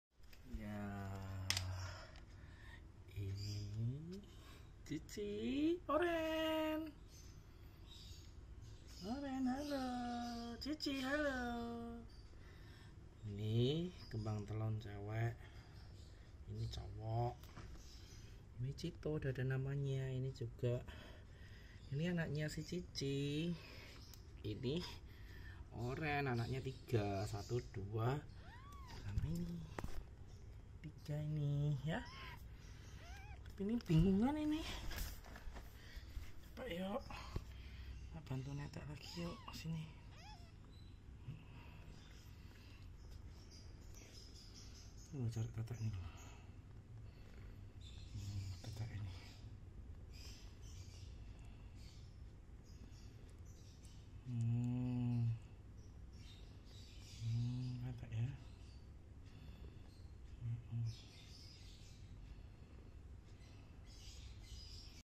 Mama berbagi susu
Nah ini dia, keseruan para simbok berbagi susu buat anak-anak mereka..
kompak sekali.
pemandangan yang tidak biasa menurutku